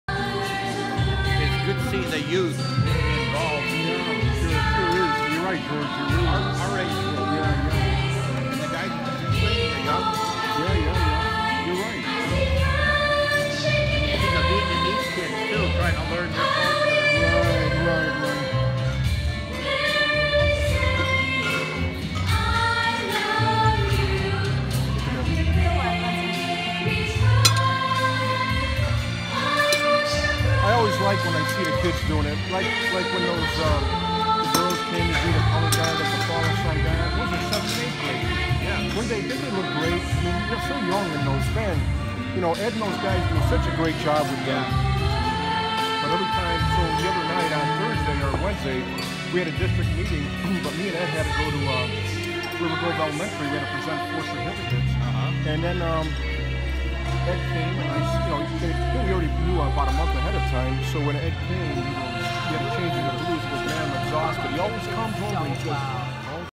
Chicago Nightingales induction ceremony, three young ladies singing.
Filmed at the Drury Lane banquet hall in Oakbrook Terrace. Three fine young ladies belting it out.